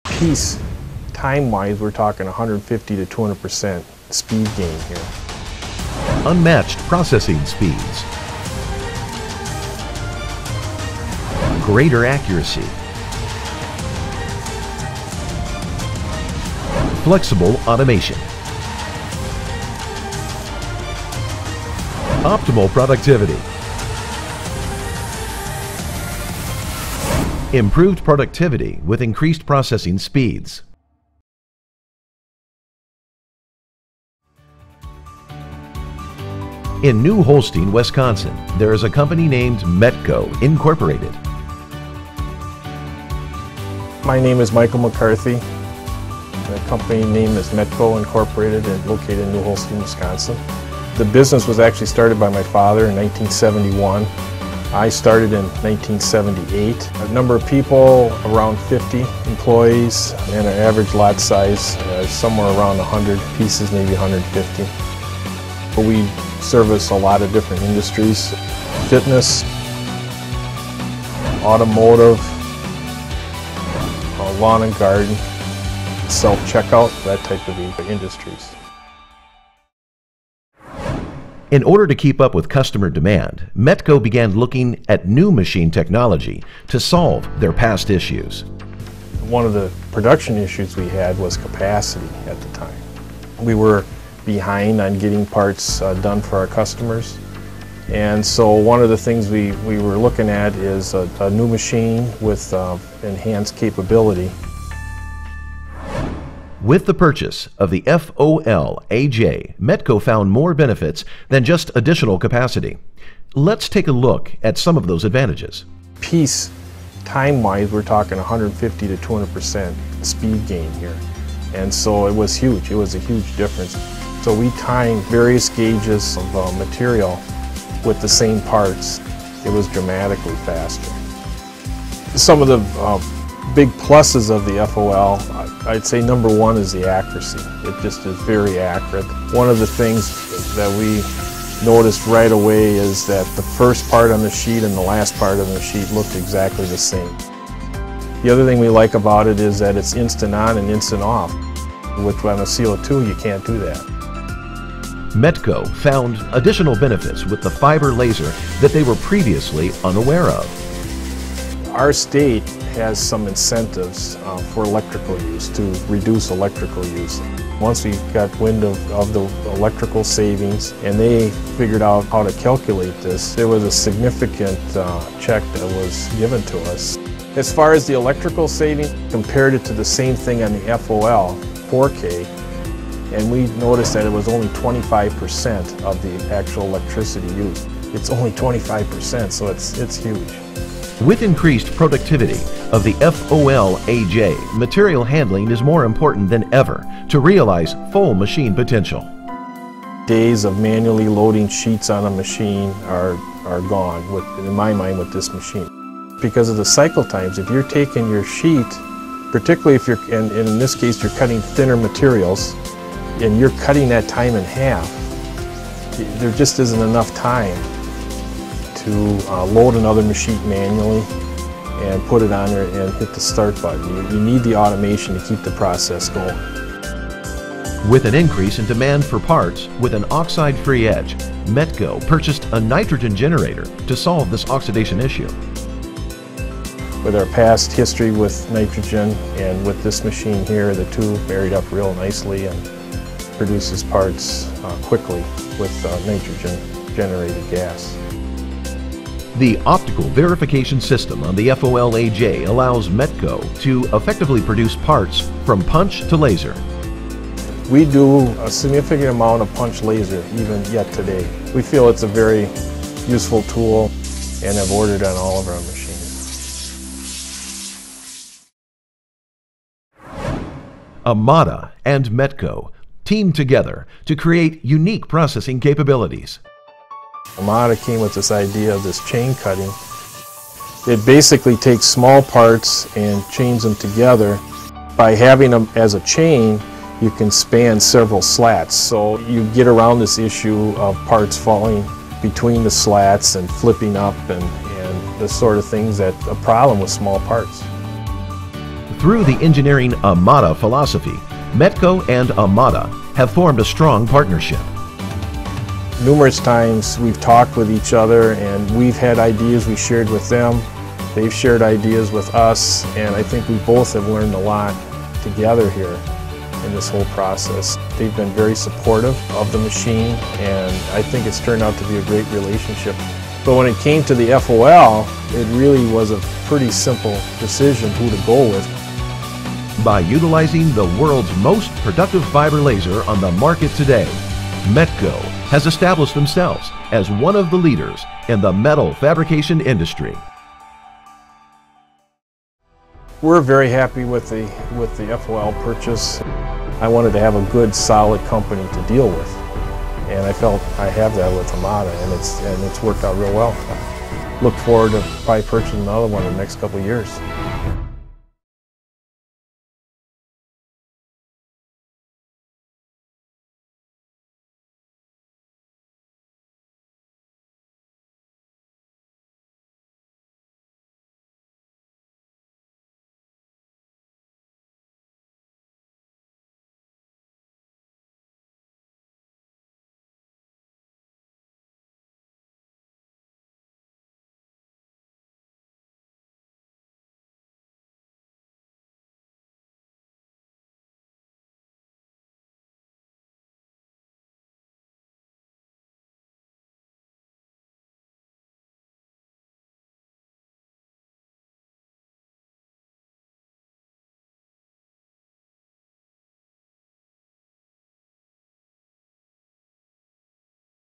Metko, Inc. — New Holstein, WI
Learn how Metko, Inc. improved productivity and increased processing speeds with the FOL 3015 AJ Fiber Laser.